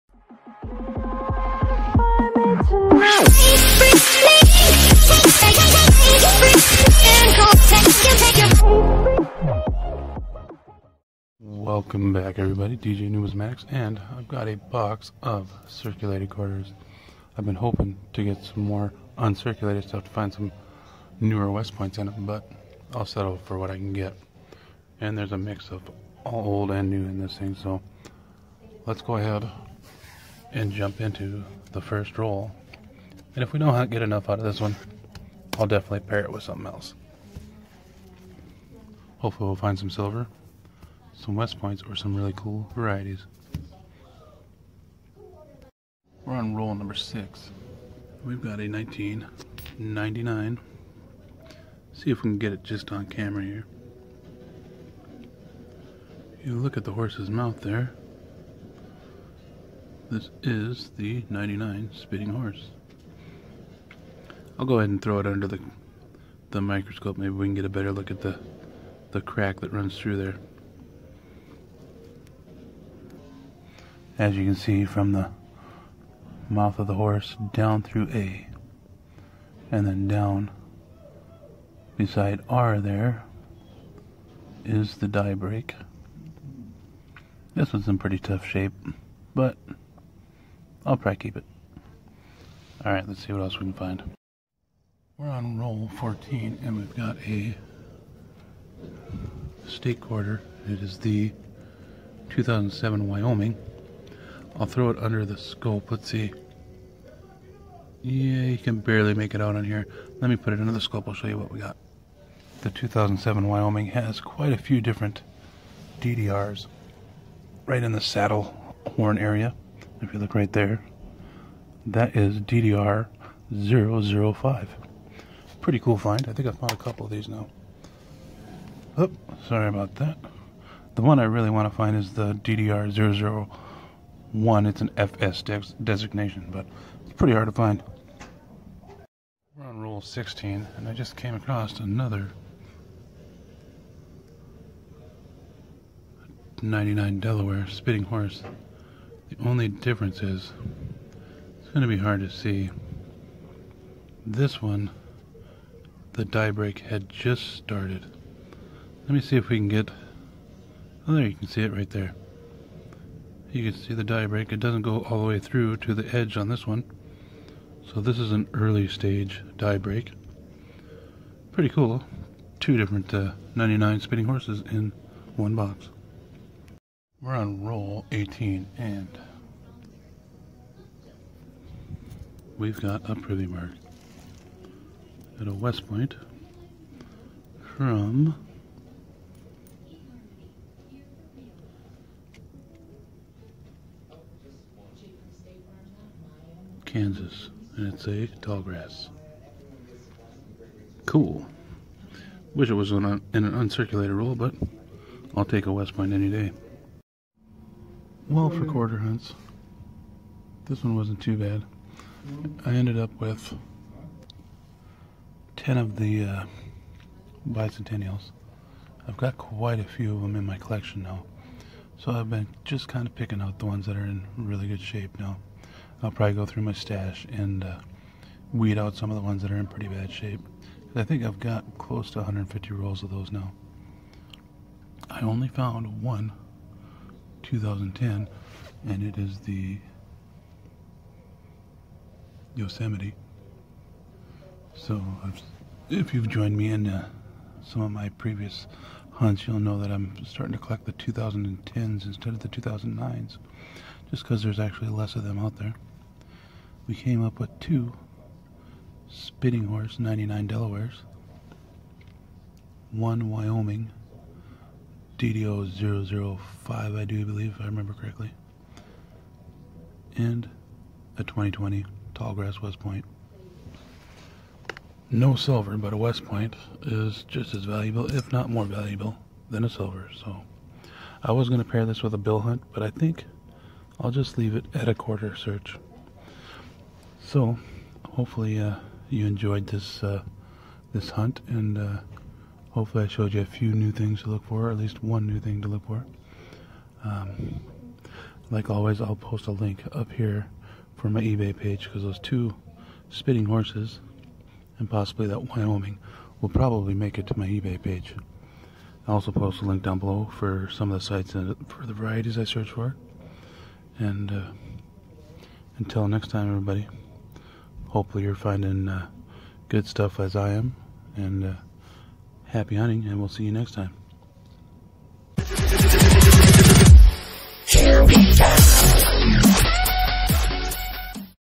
QUARTER BOX HUNT- GOOD THING I DON'T JUST LOOK FOR SILVER
PICKED UP A BOX OF CIRCULATED QUARTERS AND FINISHED THIS VIDEO TOO SOON BECAUSE I WAS ABLE TO PICK UP ANOTHER. OX. OH WELL I FOUND SOME DECENT STUFF TO SHOW YA HOPE YOU ENJOY.